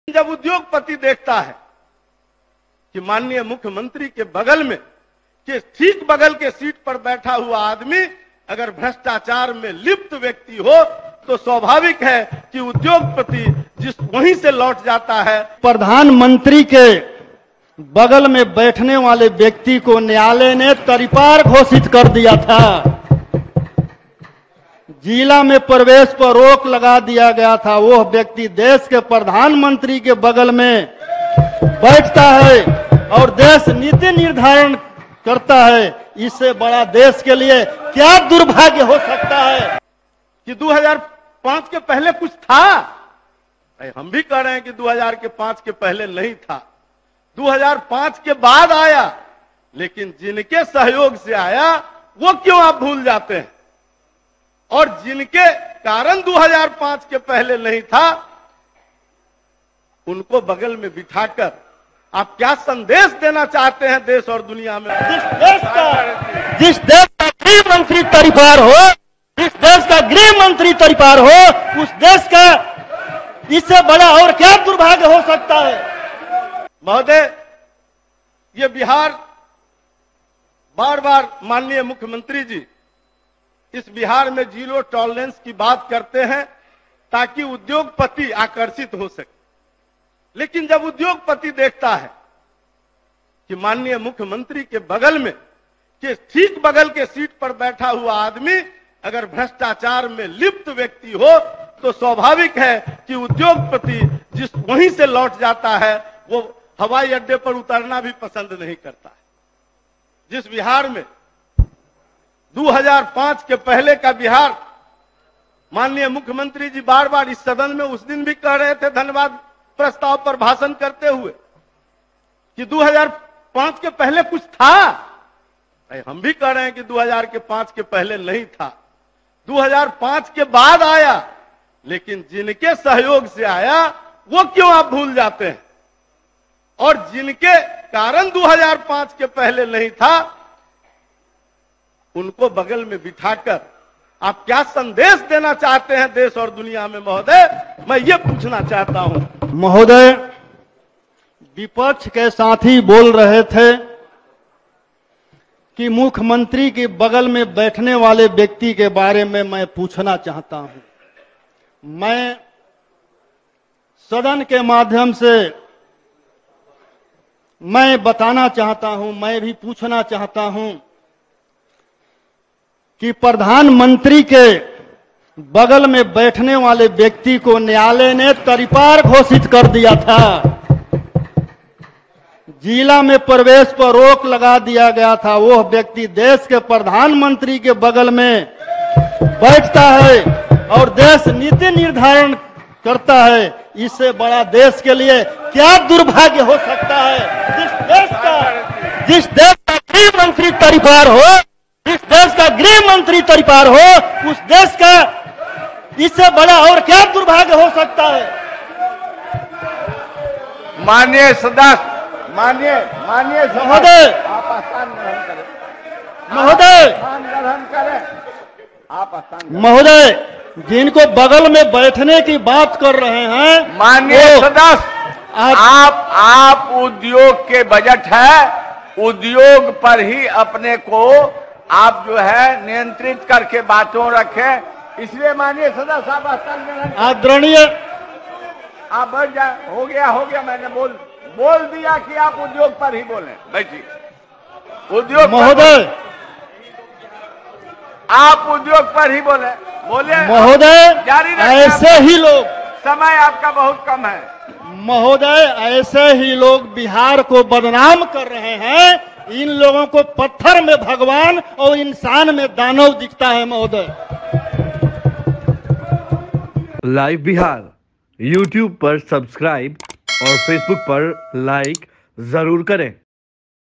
Vidhansabha में BJP ने Tejashwi को बताया भ्रष्टाचारी तो RJD ने अमित शाह को कहा तड़ीपार, सुनिए
Live Bihar Digital Network